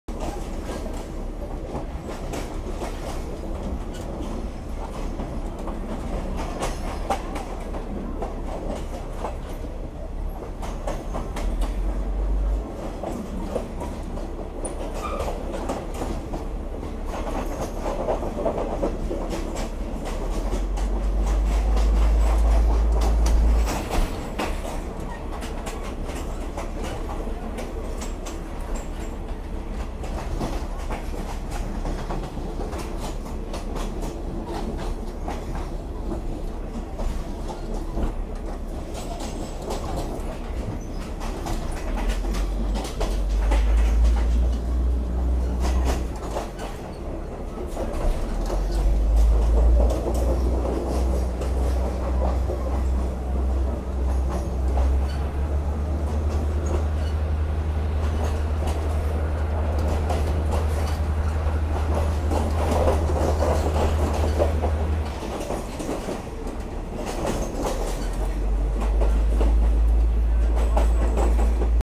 ความแรงของข้อต่อรถไฟเวลาวิ่ง
ที่ไม่ไห้ยืนตรงข้อต่อรถไฟเพราะแบบนี้ ดูความแรงเอา
ช่วงกลางๆคลิปจะโยกและเหวี่ยงแรงมาก